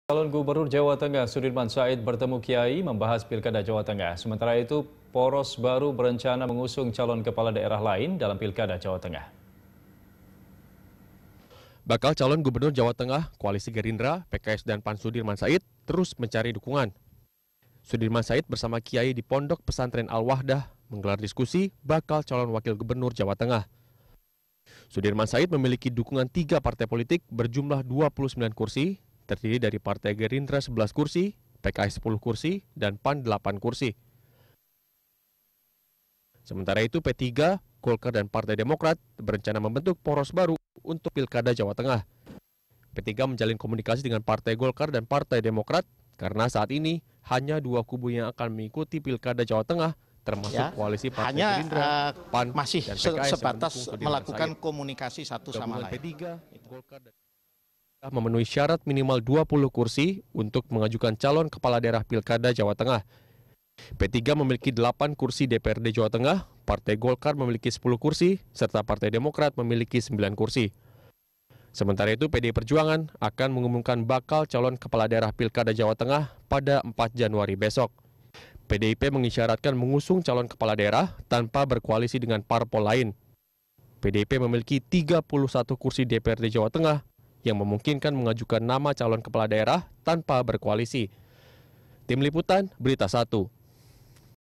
Dinamika Politik Jelang Pilkada Jawa Tengah
Bakal calon gubernur Jawa Tengah bertemu kiai membahas pilkada. Sementara itu poros baru berencana mengusung calon kepala daerah lain dalam Pilkada Jawa Tengah.

Facebook.com/BeritaSatuTV
Youtube.com/BeritaSatu